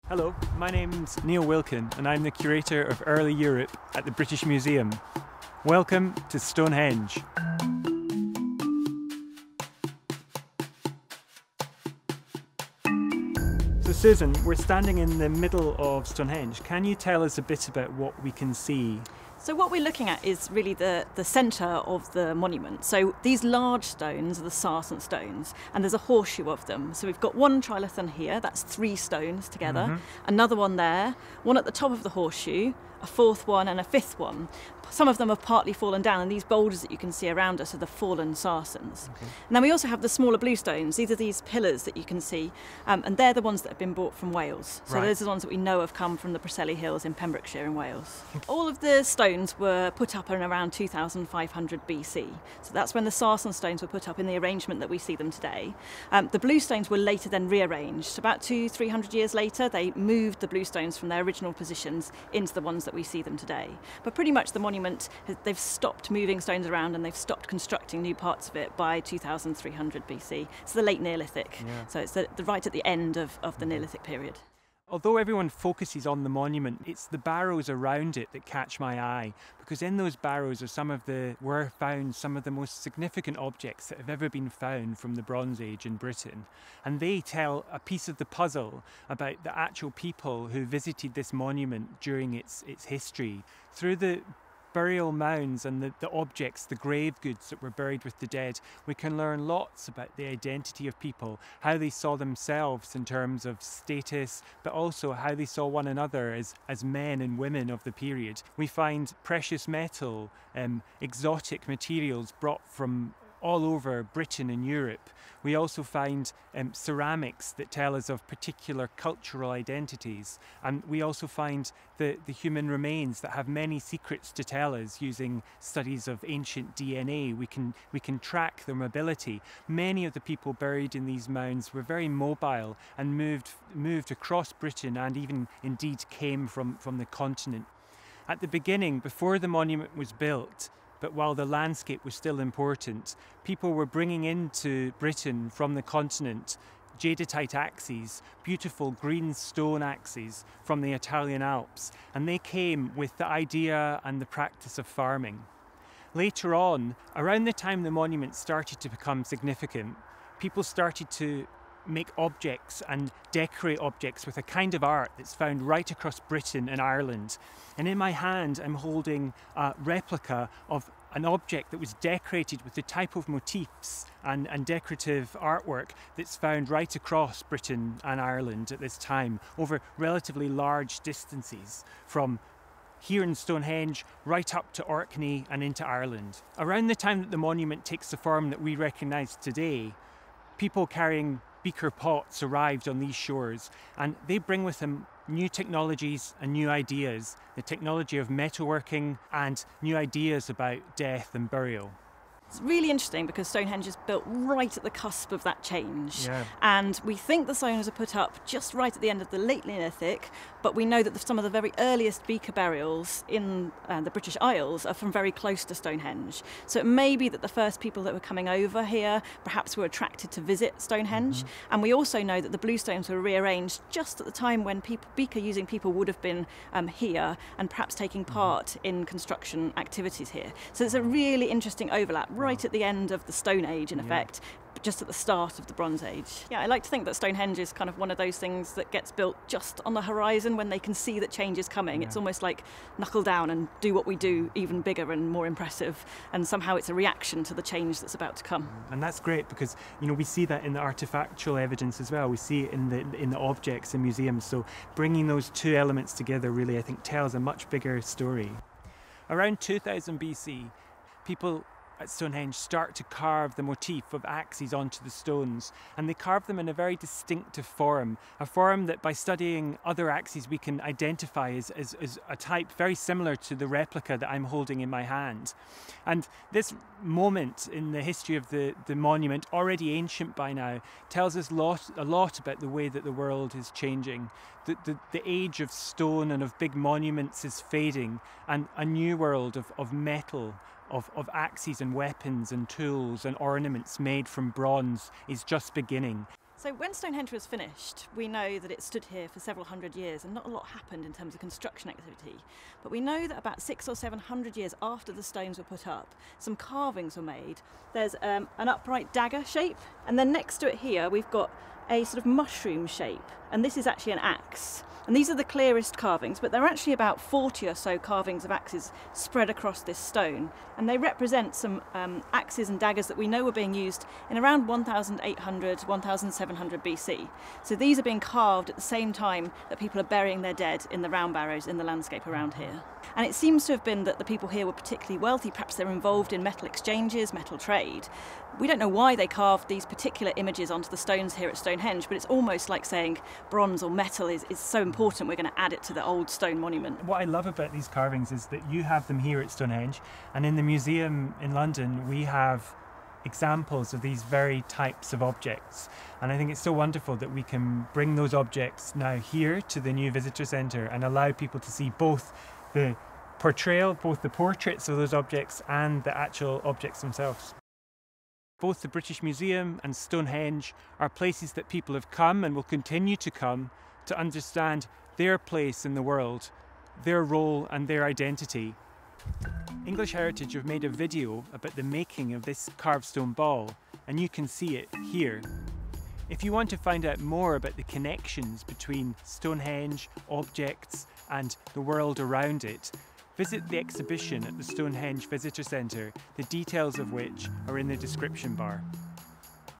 What's the meaning of Stonehenge? I Curator's Corner S4 Ep2
Neil Wilkin is back with another bronze age adventure. In this episode he is joined by Susan Greaney, Senior Properties Historian for English Heritage to discuss the history and importance of Stonehenge. Going into the heart of the monument and looking at some related bronze age objects Neil and Susan explore the connections between Stonehenge, the rest of Britain and the continent.

Made in collaboration with English Heritage check out their video about making a replica bronze age carved stone ball

Filmed on location at Stonehenge, all drone footage copyright English Heritage.